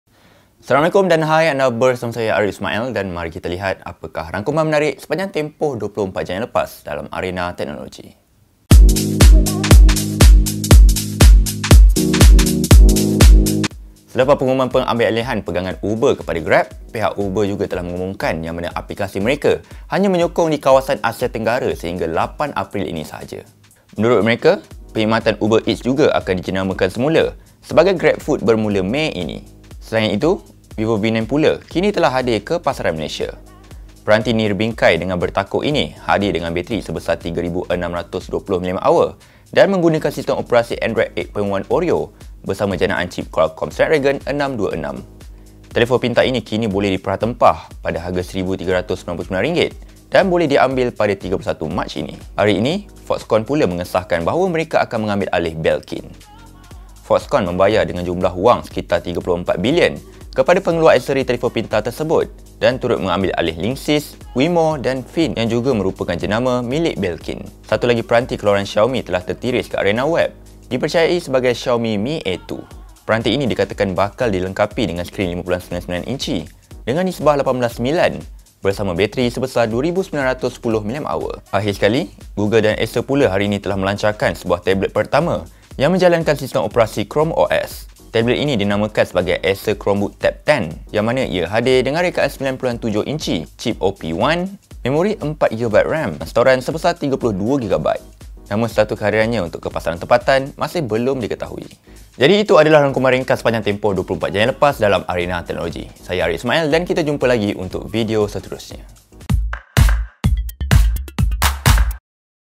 Amanz24 - UberEats Dijenamakan Sebagai GrabFood, Vivo V9 RM 1399, Foxconn Mengambil-alih Belkin
Selepas pengumuman pengambil alihan pegangan Uber kepada Grab, kini, pihak Uber juga mengumumkan bahawa aplikasi Uber hanya akan beroperasi di Asia Tengga sehingga 8 April ini sahaja.  Menurut mereka, perkhidmatan UberEats mereka juga akan dijenamakan semula sebagai GrabFood bermula Mei ini.

Selain itu, Vivo V9 pula kini telah hadir ke pasaran Malaysia. Peranti Nirbingkai dan bertakuk ini hadir dengan bateri sebesar 3620mAh dan menggunakan sistem operasi Android 8.1 Oreo bersama janaan cip Qualcomm Snapdragon 626. Telefon pintar ini kini boleh dipra tempah pada harga RM1399 dan boleh diambil pada 31 Mac ini.

Hari ini Foxconn pula mengesahkan bahawa mereka bakal mengambil alih Belkin. Faxconn membayar dengan jumlah wang sekitar RM3.4 bilion kepada pengeluar aksesori telefon pintar tersebut dan turut mengambil alih Linksys, Wemo, dan Phyn yang juga merupakan jenama milik Belkin.

Satu lagi peranti keluaran Xiaomi telah tertiris di arena web, dipercayai sebagai Xiaomi Mi A2. Peranti ini dikatakan bakal dilengkapi dengan skrin 5.99 inci dengan nisbah 18:9 bersama bateri sebesar 2910 mAh.

Akhir sekali, Google dan Acer pula hari ini telah melancarkan sebuah tablet pertama yang menjalankan sistem operasi Chrome OS. Tablet ini dinamakan sebagai Acer Chromebook Tab 10 dimana ia hadir dengan rekaan skrin 9.7 inci, cip OP1, memori 4GB RAM dan ruangan storan sebesar 32GB. Namun status kehadirannya ke pasaran tempatan masih belum diketahui.